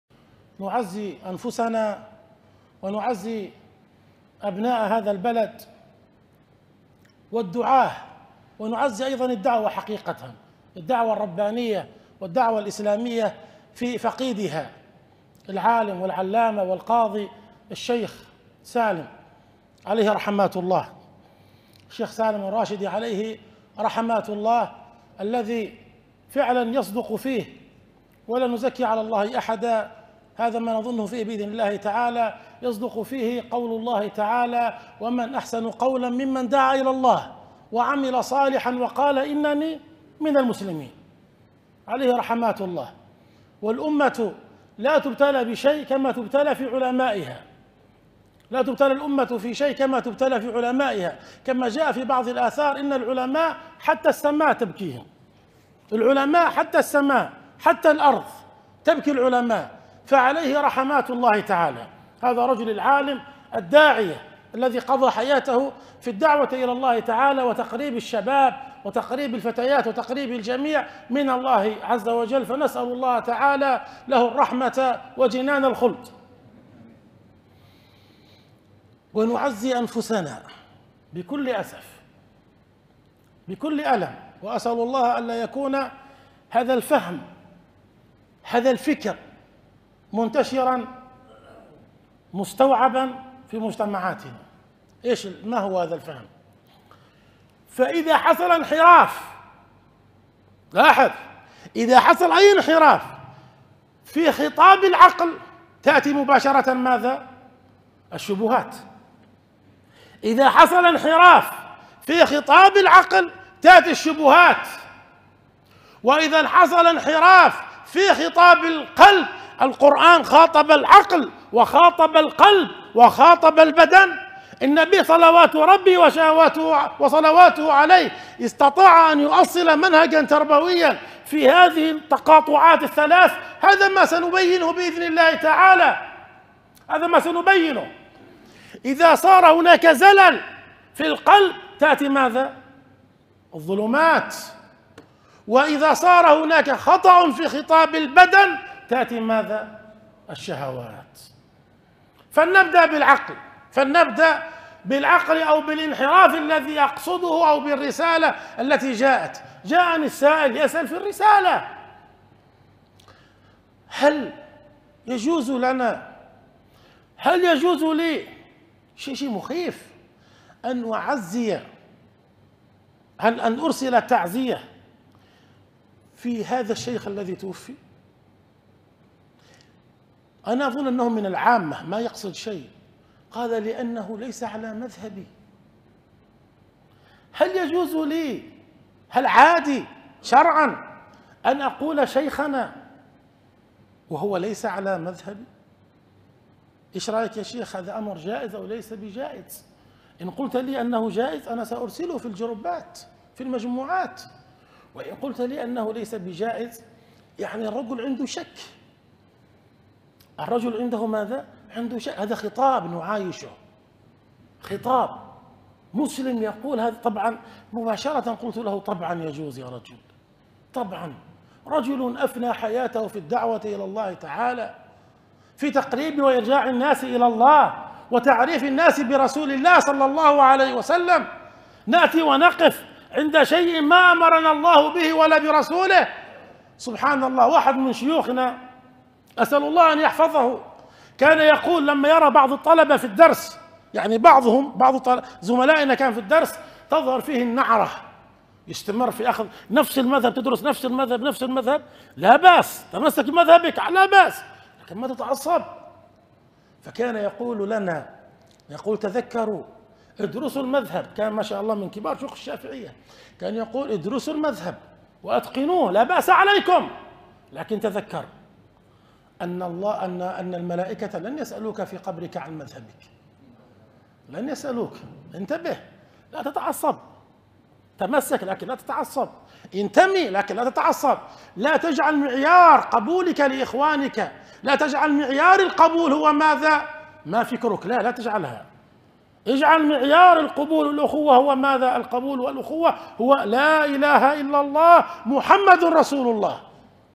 الشيخ عبدالله الشحري حفظه الله يعزي الشعب العماني في وفاة الشيخ سالم الراشدي رحمه الله تعالى
مصدر المقطع:

درس السيره النبوية 12

الباحث الشيخ عبدالله الشحري